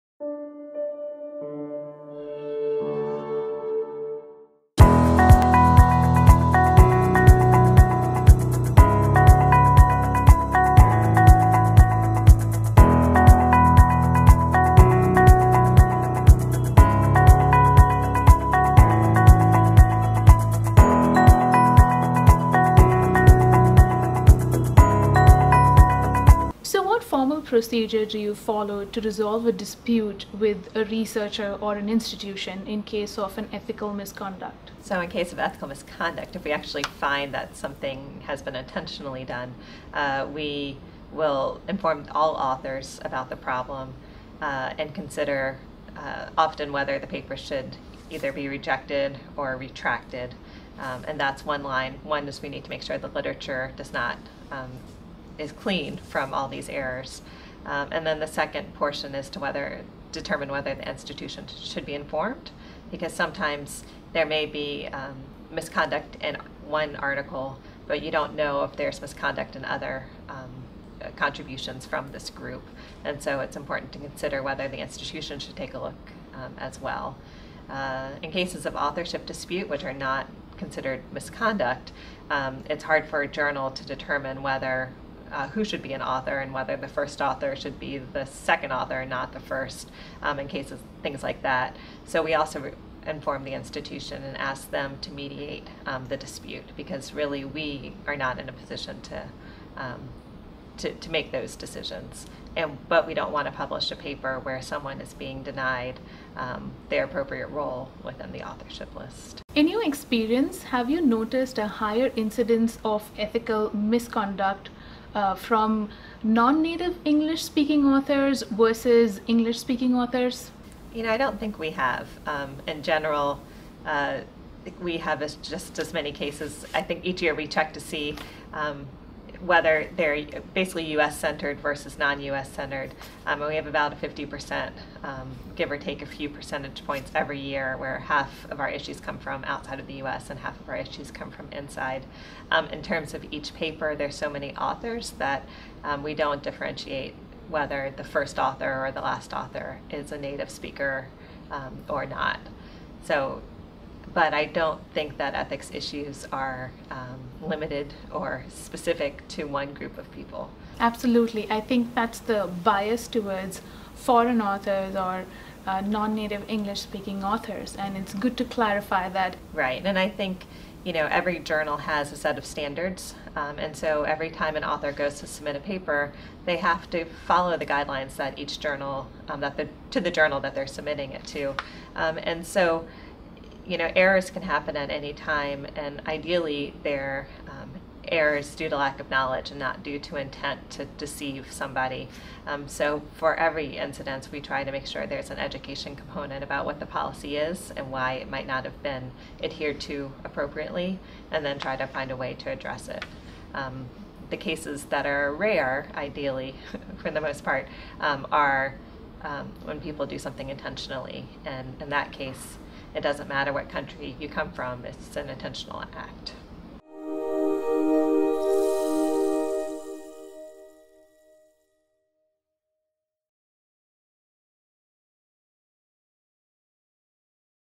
Identifying Ethical Misconduct - An Interview with Christina Bennett : Part 6
In this interview series, we will take you through the various issues that the American Physiological Society follows to overcome the editorial and ethical challenges faced by their journals. Christina shared some thoughts on how APS continues to improve its ethical guidelines and how they resolve disputes between researchers, peer reviewers, and institutions in cases where ethical misconduct has been reported. Christina also shares some details about her involvement at CSE and highlights of her session on “Detecting Misconduct in Images” at the 2016 ISMTE conference at Philadelphia, PA. She also shares some useful tips that would help early-career professionals avoid misconduct and discuss some future challenges in identifying ethical and research misconduct in the coming decades.